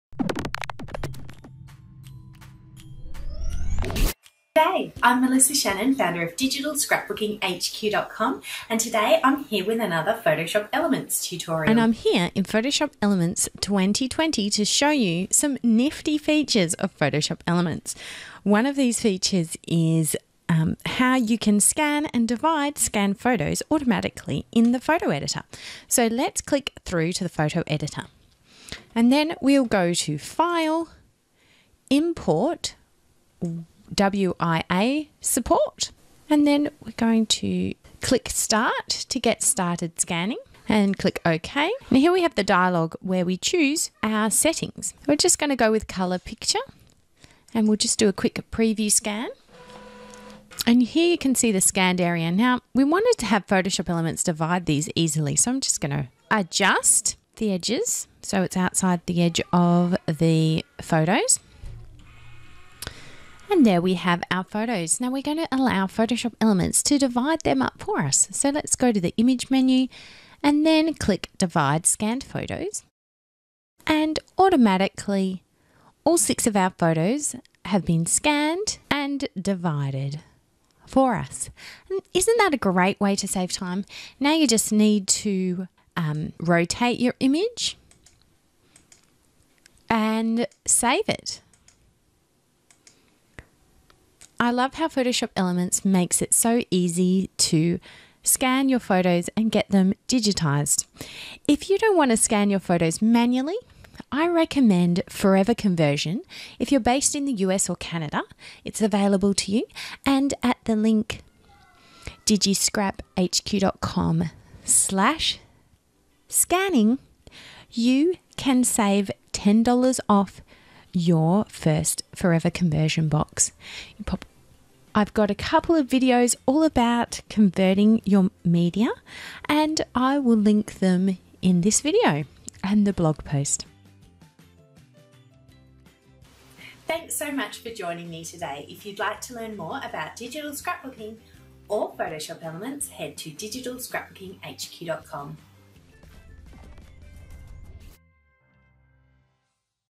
How to Divide Scanned Photos in Photoshop Elements
When you have a lot of small vintage photos to scan, you can save time by laying them all out on your flatbed scanner.

As long as the edges are not touching, Photoshop Elements can split them up for you! (This feature is available in older versions of PSE too.)

Simply scan your photos
Open the resulting file in Photoshop Elements Editor
Click on the Image menu, then Divide Scanned Photos... and Photoshop Elements will do the work for you!
Use Image - Rotate menu to rotate the photos if needed
It works best with photos where the edges are visible and the photos are all the same size, but it will certainly speed up your scanning now you don't have to scan them one by one!

Happy scanning!
Melissa